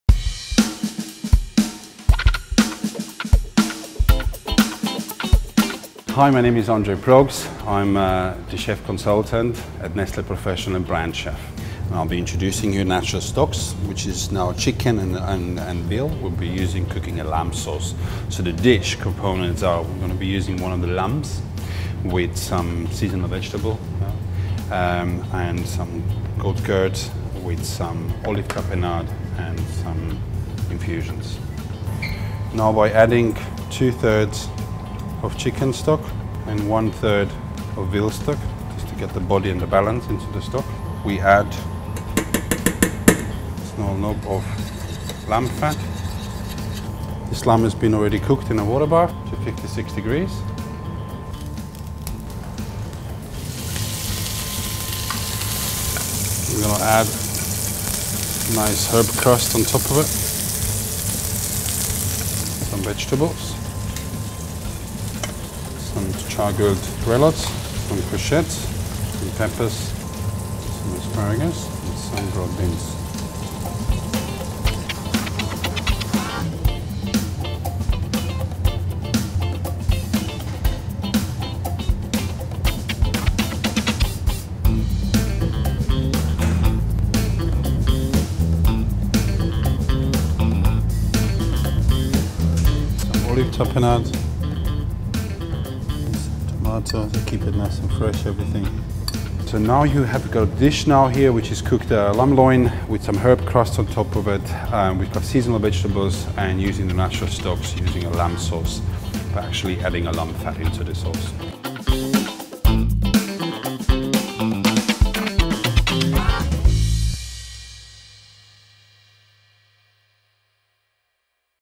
Lamb with herb crust, summer vegetable and olive tapenade potato recipe
Lamb with herb crust, summer vegetable and olive tapenade potato recipe. Nestle CHEF development chef Andrej Prokes formerly of Michelin Guide UK 3-Star The Fat Duck and Michelin Guide UK listed The Ritz, creates this dish with an easy to follow step by recipe. for more great lamb recipes go

Category
Howto & Style
License

Watch the world's best chefs for free today. Network, add and share images, recipes, jobs and so much more.